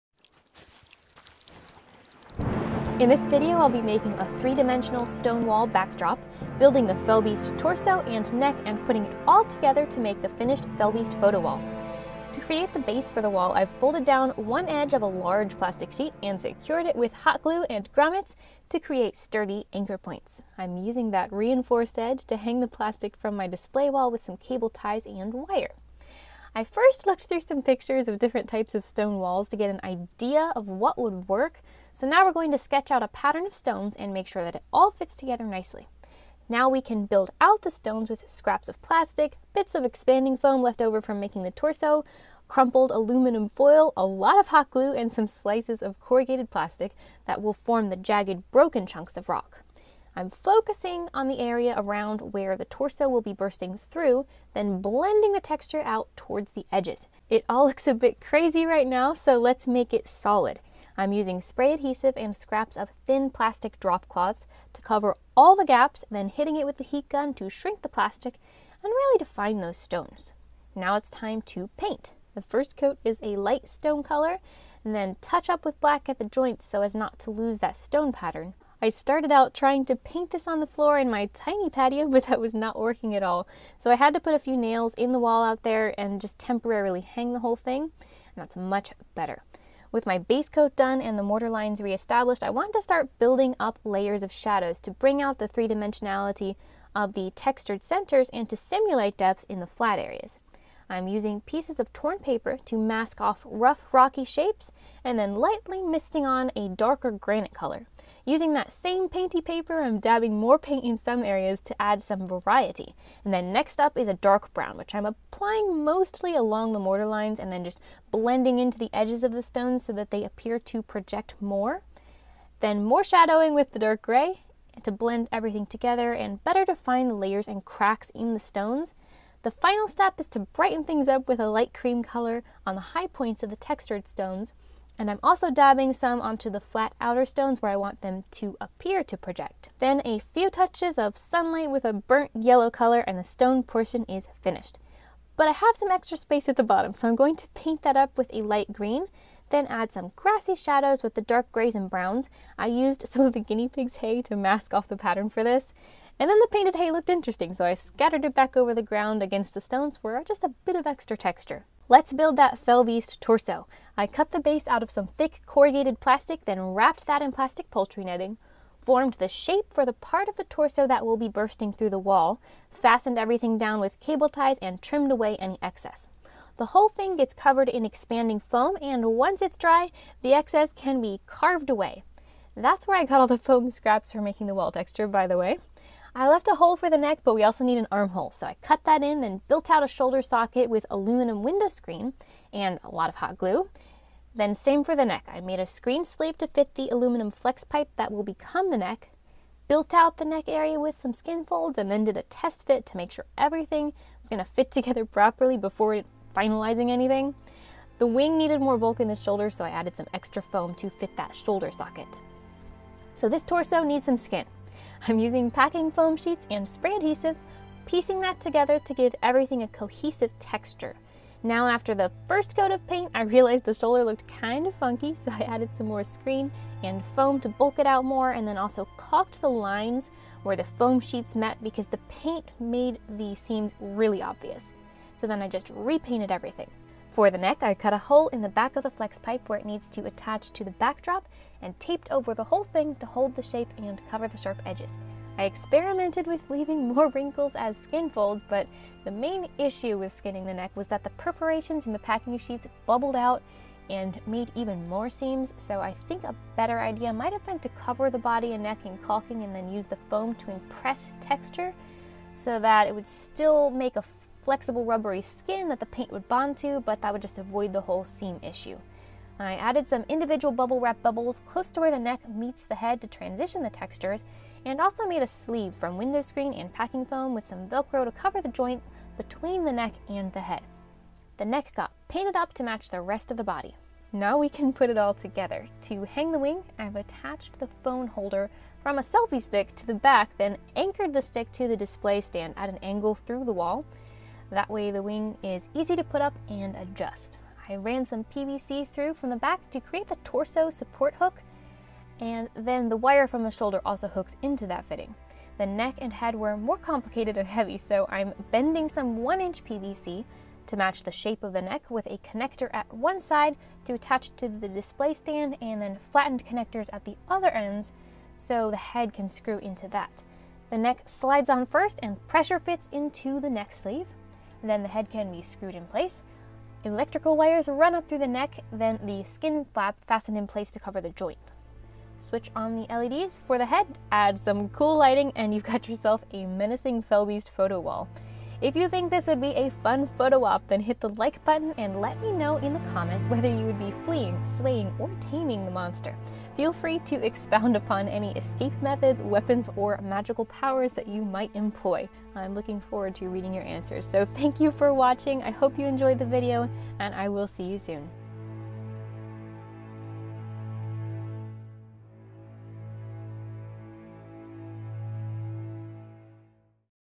Building a Castle Wall Backdrop | LOTR Fell Beast of Mordor Photo Booth Part 3
Sculpting and painting a textured stone backdrop, building the Nazgul fell beast torso and neck and putting it all together with the wing and head to create the finished photo wall.

| Supplies |
Large Plastic Sheet
Plastic Poultry Netting
Expanding Foam
Aluminum Window Screen
Foam Wrap Roll (Skin)
Spray Adhesive
8" x 10 ft. Aluminum Flex Pipe
Flat Black Spray Paint
Satin Stone Gray Spray Paint
Satin Granite Spray Paint
Camouflage Earth Brown
Camouflage Sand Spray Paint
Satin Classic Navy Spray Paint
Flat Red Primer
Flat Dark Gray Primer
Matte French Cream

Equipment Used
Canon Powershot G7X
Zoom H1 Recorder
Giant Squid Audio Lab Mic